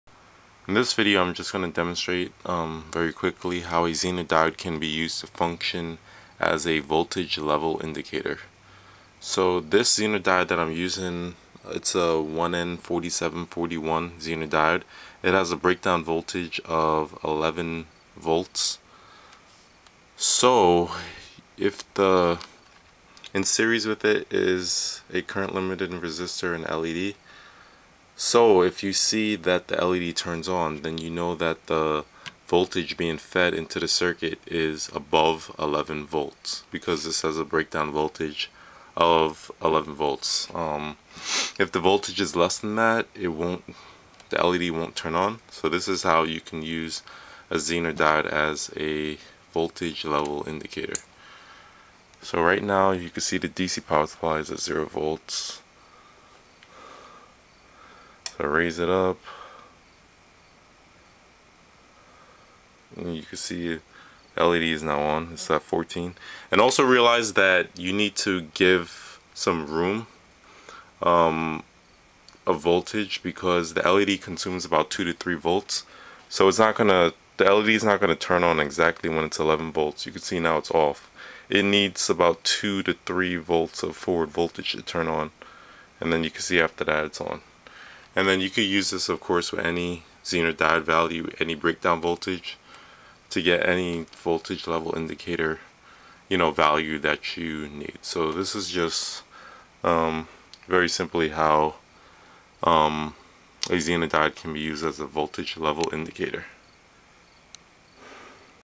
Zener Diode Voltage Level Indicator
This video demonstrates a zener diode voltage level indicator circuit.

To see the full detail of this circuit, including the schematic diagram,